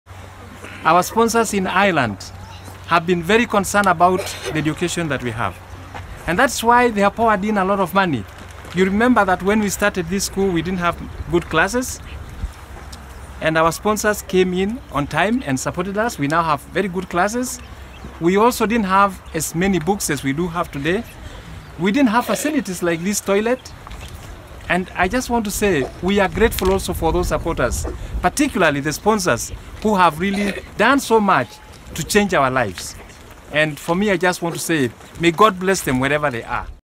Thank You Irish Sponsors!
A thank you message to Irish child sponsors from World Vision Area Programme Manager Solomon Munke